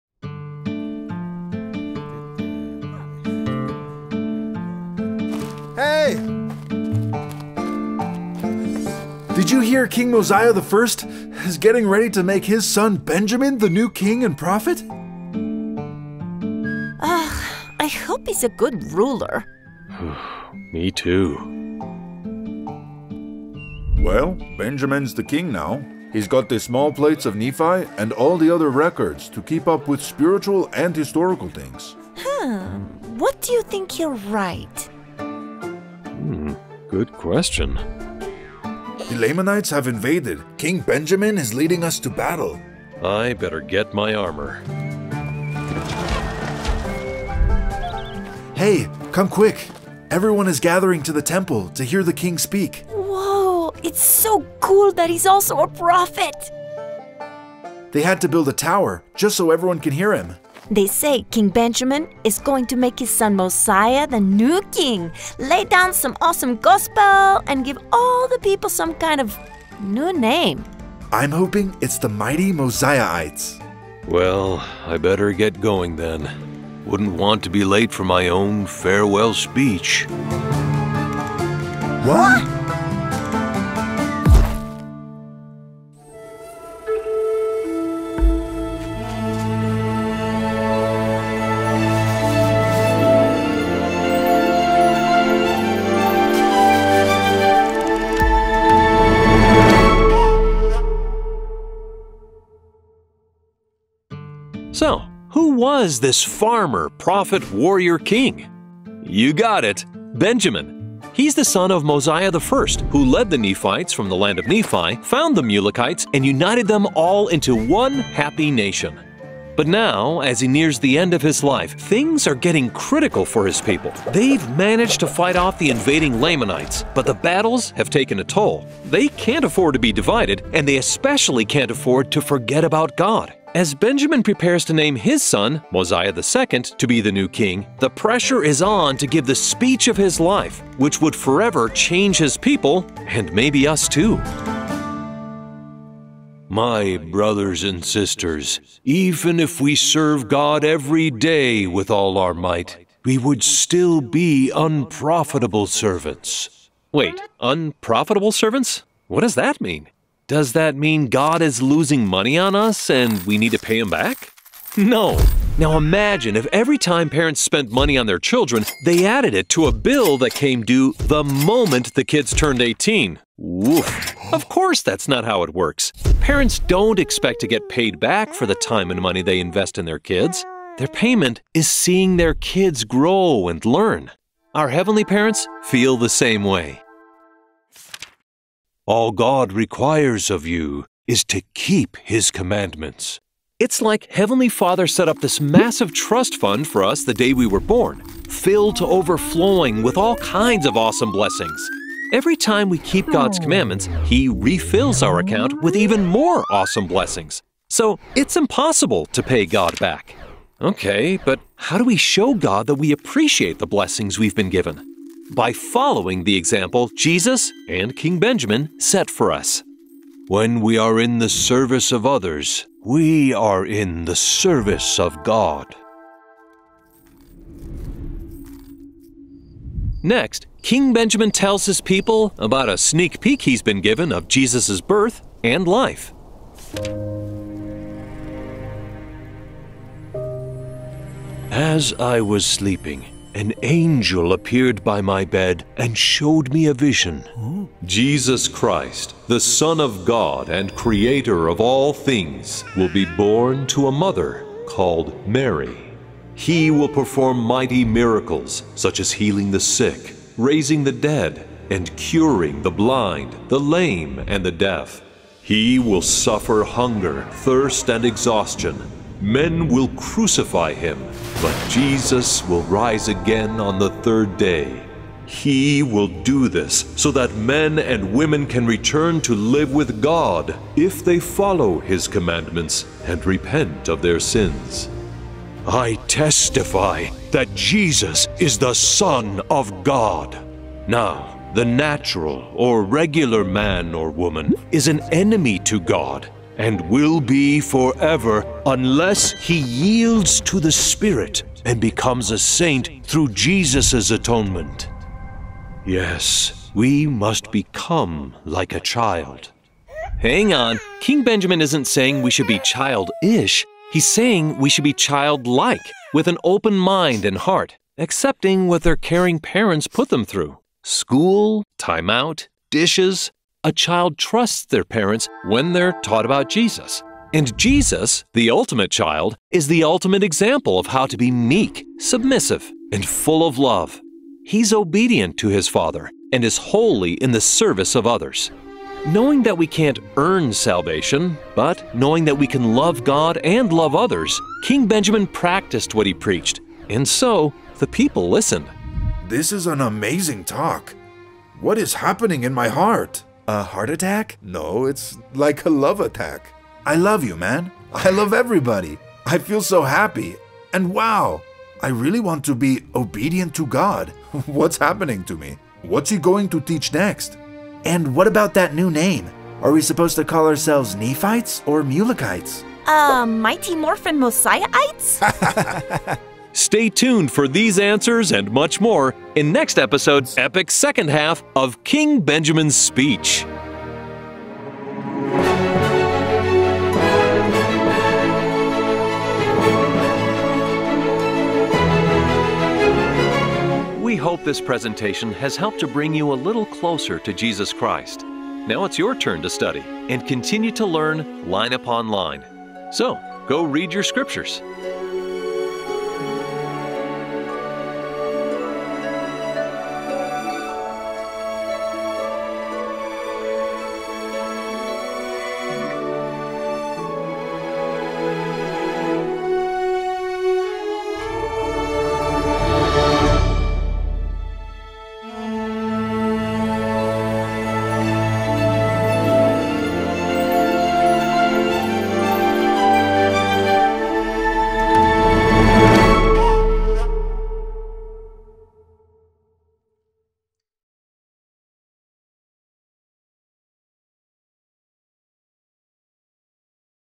Come Follow Me (April 22 - 28) The Unprofitable Servant: Mosiah 1-3
This week's Come Follow Me video is brought by Living Scriptures Streaming